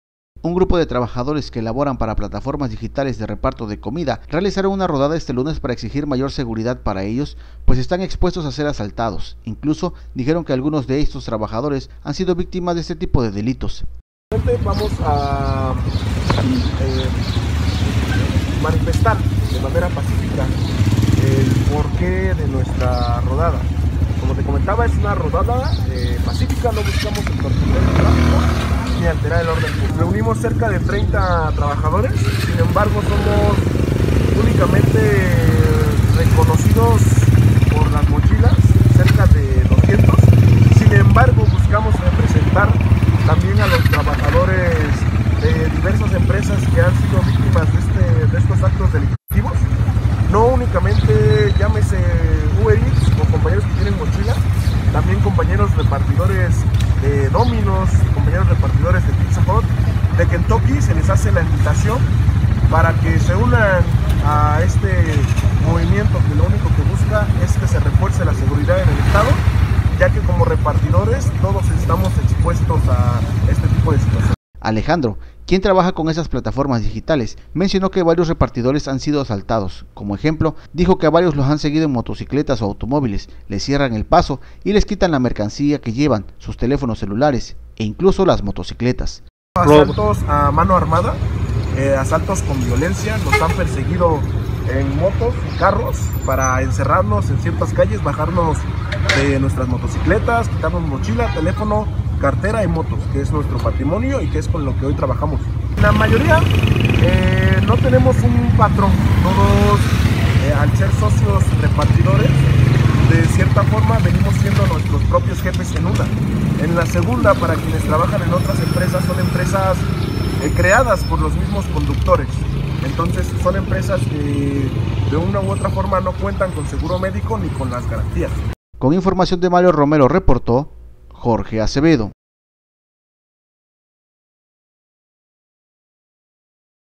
Protestan repartidores de comida en la ciudad de Oaxaca, exigen mayor seguridad
🔹Un grupo de trabajadores que laboran para plataformas digitales de reparto de comida, realizaron una rodada este lunes para exigir mayor seguridad para ellos, pues están expuestos a ser asaltados, incluso dijeron que algunos de estos trabajadores han sido víctimas de este tipo de delitos.